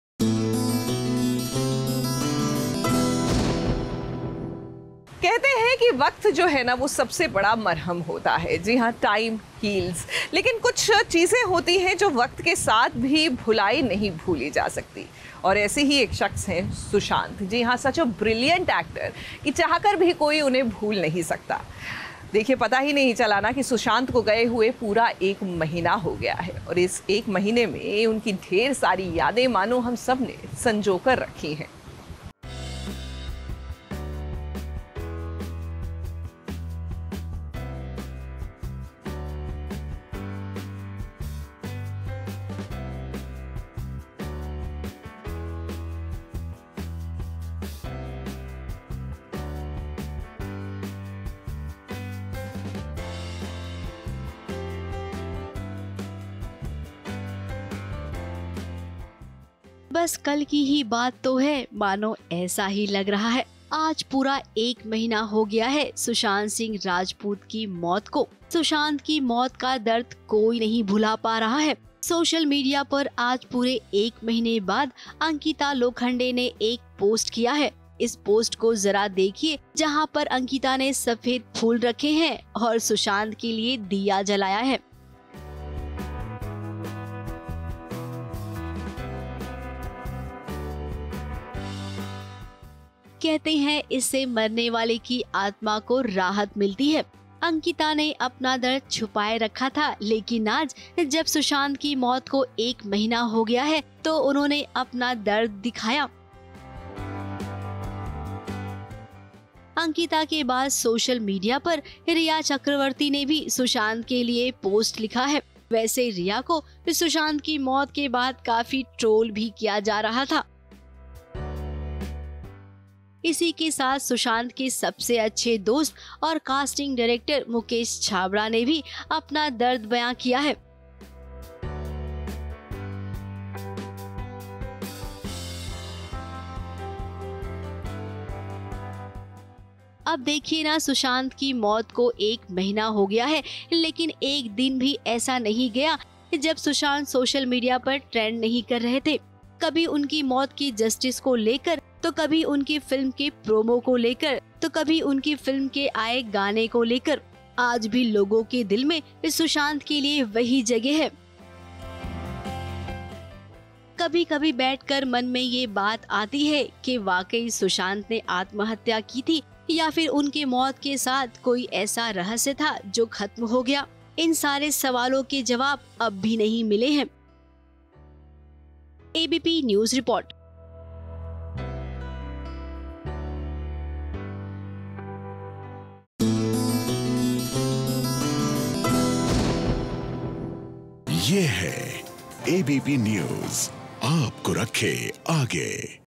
One Month After Sushant's Death, Ankita Lokhande & Rhea Chakraborty Share Feelings
One month after Sushant Singh Rajput's death, Ankita Lokhande & Rhea Chakraborty shared their feelings on social media.
Ankita posted a picture for the first time after Sushant's demise, of 'Diya' and captioned it 'Child Of God'
Rhea wrote a long paragraph and described her feelings with some pictures.